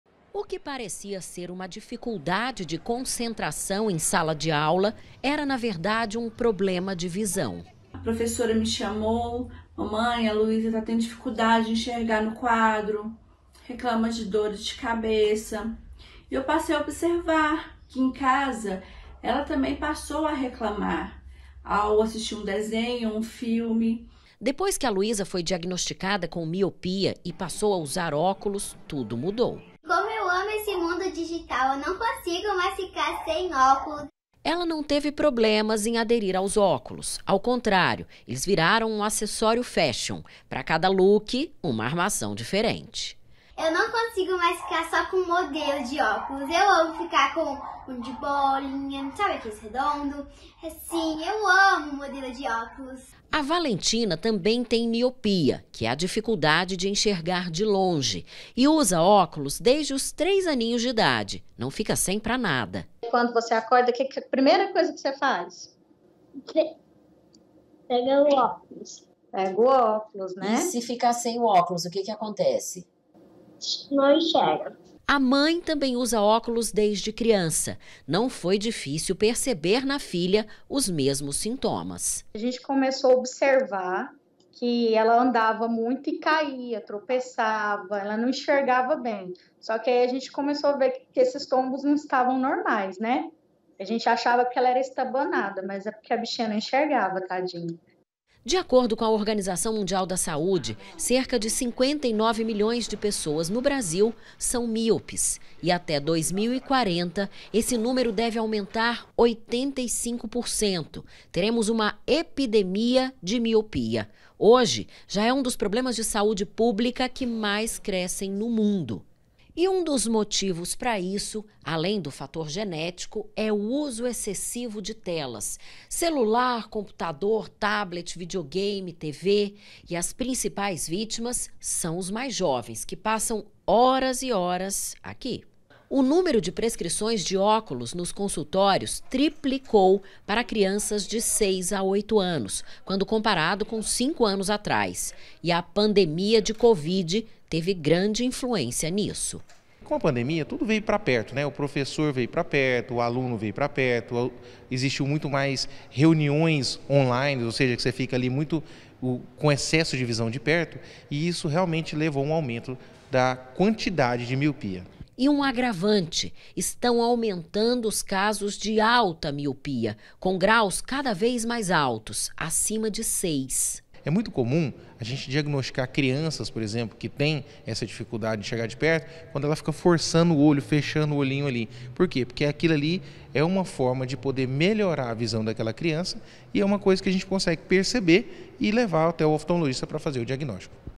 EPIDEMIA DE MIOPIA: CASOS TRIPLICAM ENTRE CRIANÇAS DURANTE A PANDEMIA
CASOS DE MIOPIA ENTRE CRIANÇAS TRIPLICARAM DURANTE A PANDEMIA. RESULTADO DAS AULAS ON-LINE E DO USO DE ELETRÔNICOS EM EXCESSO. E A SITUAÇÃO TENDE A PIORAR. A PREVISÃO É QUE O NÚMERO DE MÍOPES NO PAÍS CRESÇA 85% ATÉ 2040.